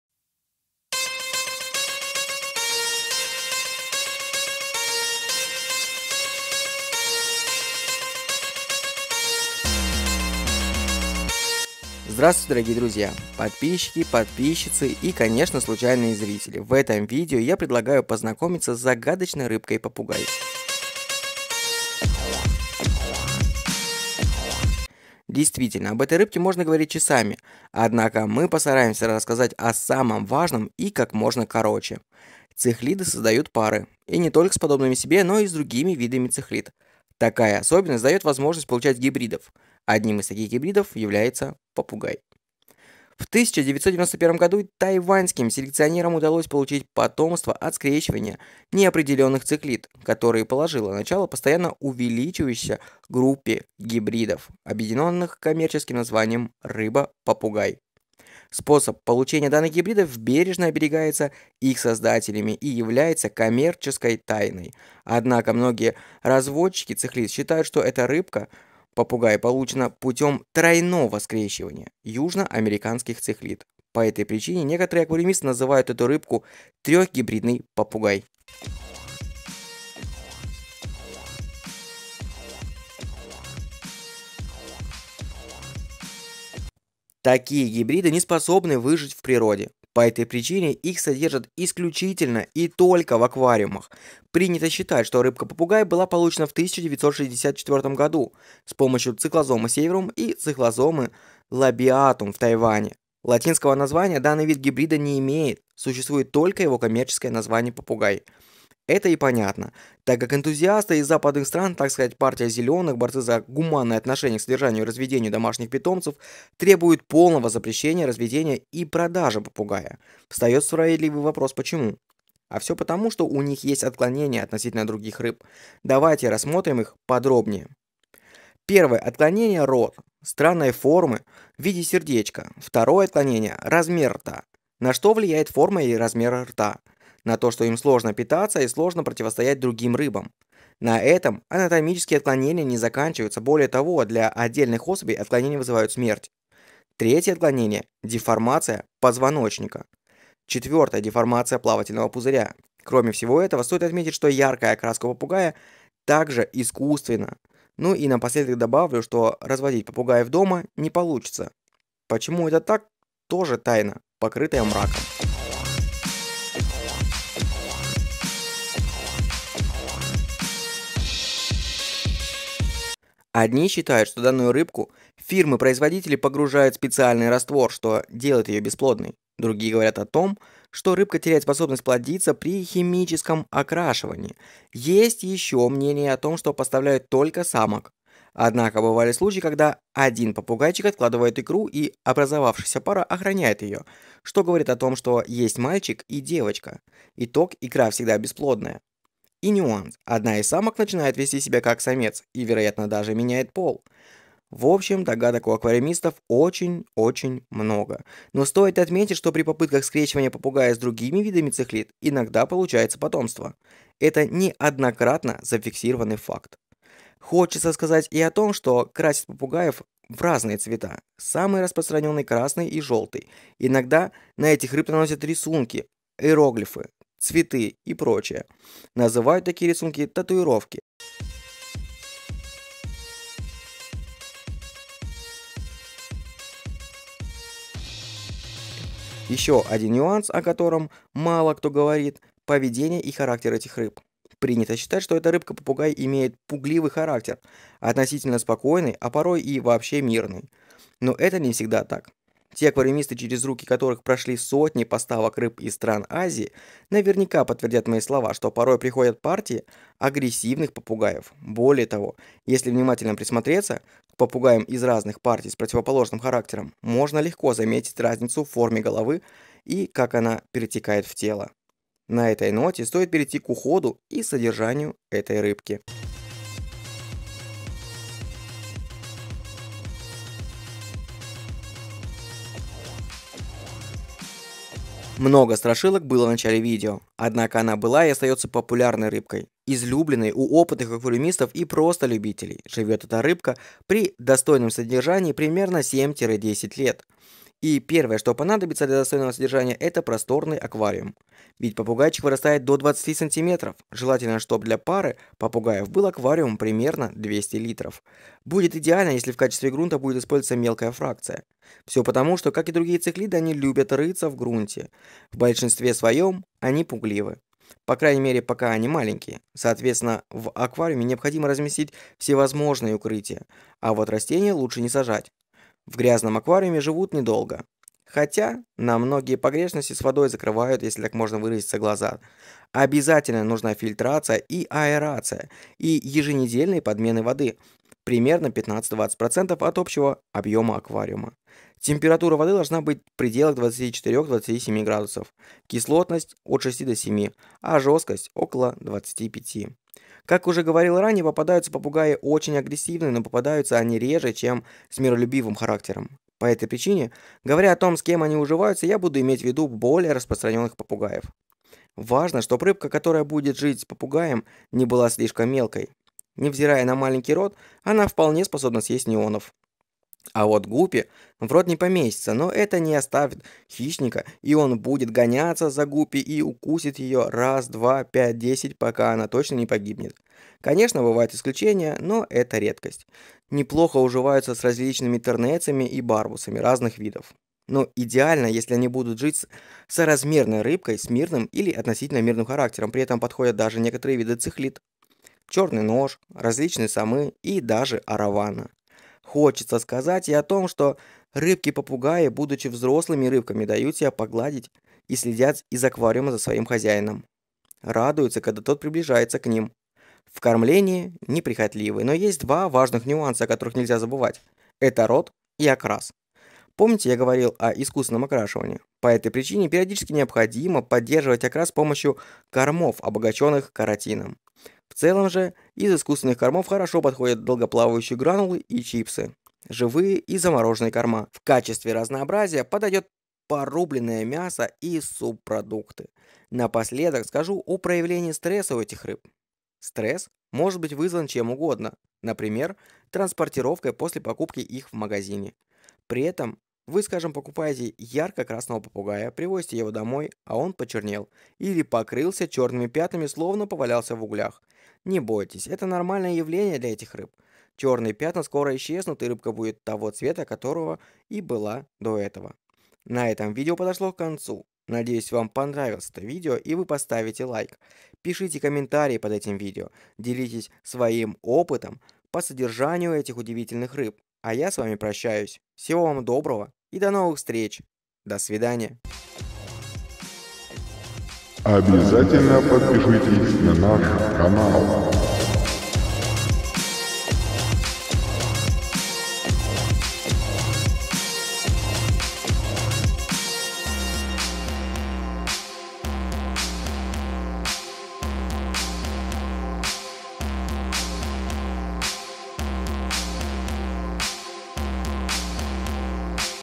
Аквариумные рыбки.Рыба попугай.Parrot Cichlid
webmoney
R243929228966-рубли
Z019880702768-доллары
Номер карты СБЕРБАНК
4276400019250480
Для рыб-попугаев следует купить аквариум длиной не менее 70 см. Если у вас имеется пара рыб, то оптимальным вариантом станет их содержание в аквариуме объёмом 150–200 литров, обязательно с крышкой, чтобы рыбы случайно не выпрыгнули из него. Внутри обязательно поместите сооружения, в которых можно укрыться, например, пещеры ,гроты, крупные камни.